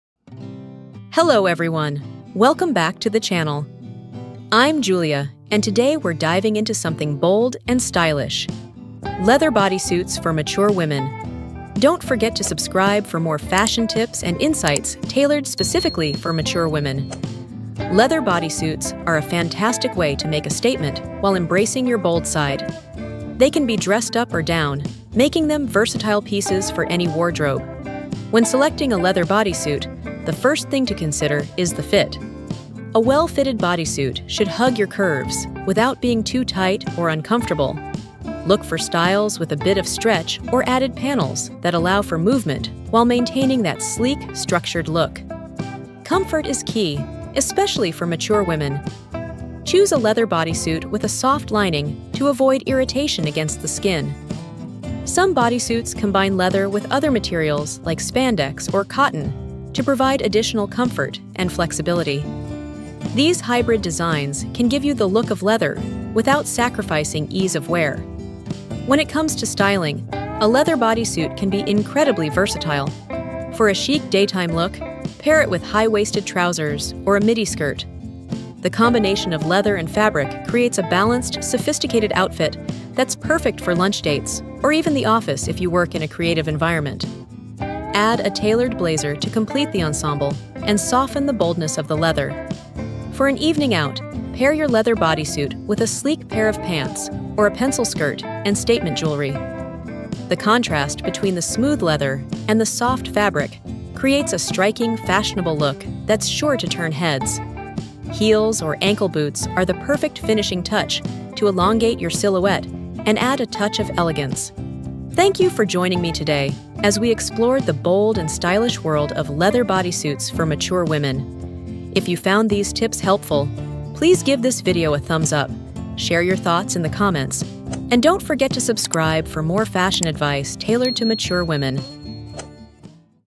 Natural Older Women Over 50 - Attractively Dressed Classy - Fashion Tips
Explore the bold and sophisticated world of leather bodysuits with Julia. Learn how to choose the perfect fit, style them for any occasion, and care for your leather pieces. Perfect for mature women looking to add an edge to their wardrobe.